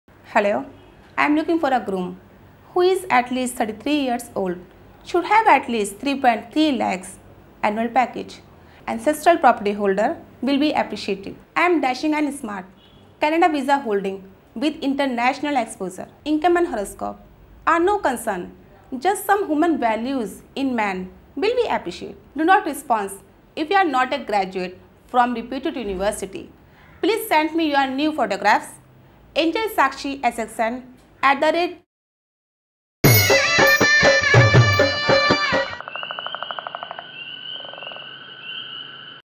Sahi Rishta Matrimonial: Sakshi #3628
Groom wanted for woman, smart, dashing, holding Canada Visa. Groom should be 33 yrs old, 3.30 lac p.a. Graduate. Ancestral property holder shall be appreciated. Income and horoscope are no concern, just some humane values.